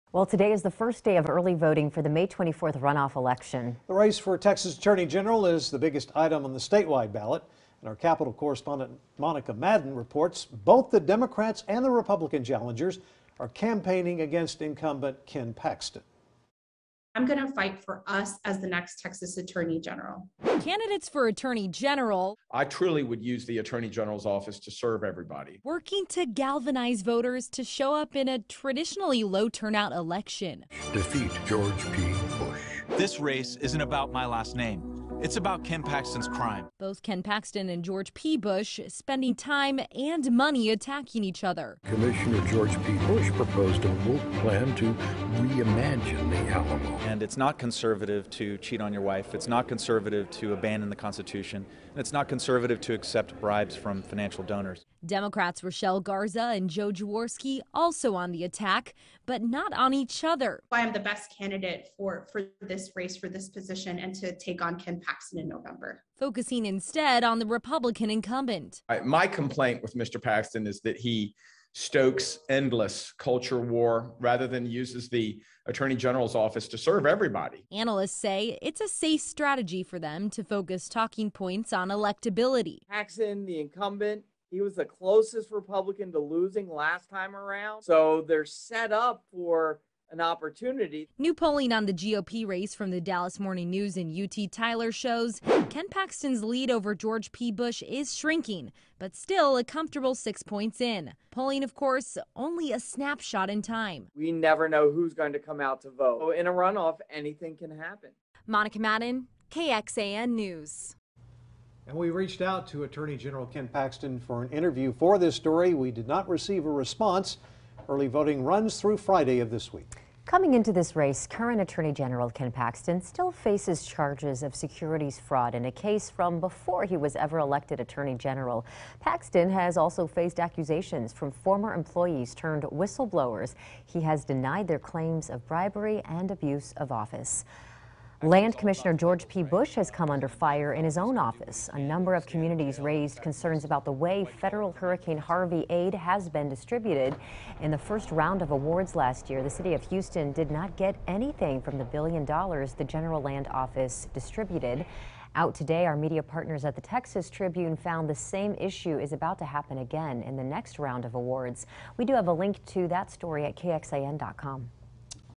Early voting for attorney general runoffs begins Monday
KXAN's Monica Madden reports that early voting for attorney general runoffs starts Monday, and none of the candidates are playing nice.